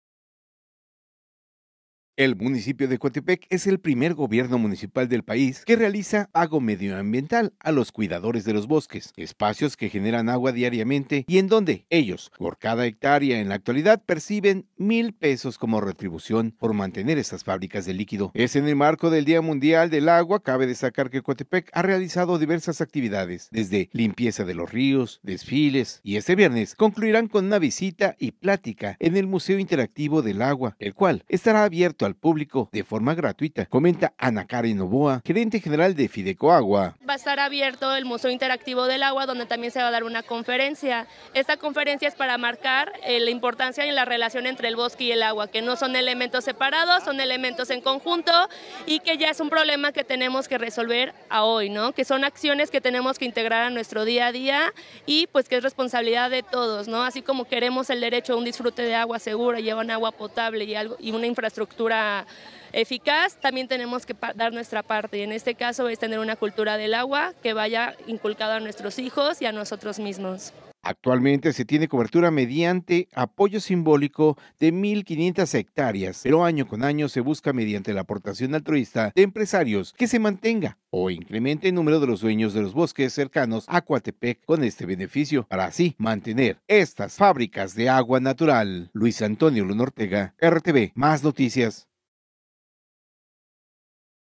Coatepec, único municipio en realizar pago medioambiental a cuidadores de los bosques
Coatepec, Ver.- El municipio de Coatepec es el primer gobierno municipal del país que realiza pago medioambiental a los cuidadores de los bosques, espacios que generan agua diariamente y en donde ellos por cada hectárea en la actualidad perciben mil pesos como retribución por mantener estas fábricas de líquido en los bosques de niebla de la región.

“Ya lleva 21 años funcionando el pago comenzó en 2002 siendo de 380 pesos por hectárea y a 2022 que fue el año pasado se tenía una suma de 1000 pesos por hectárea”.

En el marco del Día Mundial del Agua, cabe destacar que Coatepec ha realizado diversas actividades desde limpieza de los ríos, desfiles y este viernes concluirán con una visita y plática en el Museo Interactivo del Agua, el cual estará abierto al público de forma gratuita comentó, Ana Karen Novoa, gerente general de Fideocoagua.

“No es un programa de apoyo, es un programa de pago por servicios ambientales, por eso reiteramos la importancia de cubrir nuestros costos, pagar mensualmente a la Comisión de Agua y Saneamiento municipal, porque de ahí se recauda el dinero para Fideicoagua”.

  Actualmente se tiene la cobertura mediante apoyo simbólico de mil quinientas hectáreas, pero año con año se busca, mediante la aportación altruista de empresarios, que se mantenga el número de dueños de los bosques cercanos a Coatepec para mantener esta fábrica de agua natural.